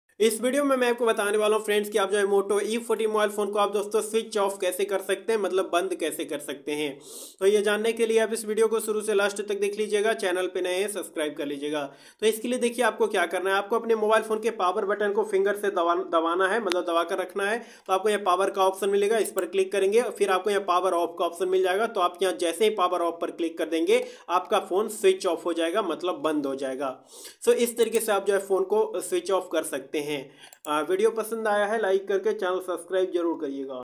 moto e40 phone switch off kaise kare । moto e40 power off।how to switch/power off moto e40 phone
is video me bataya gaya hai friends ki moto e40 mobile ko switch off kaise kiya jata hai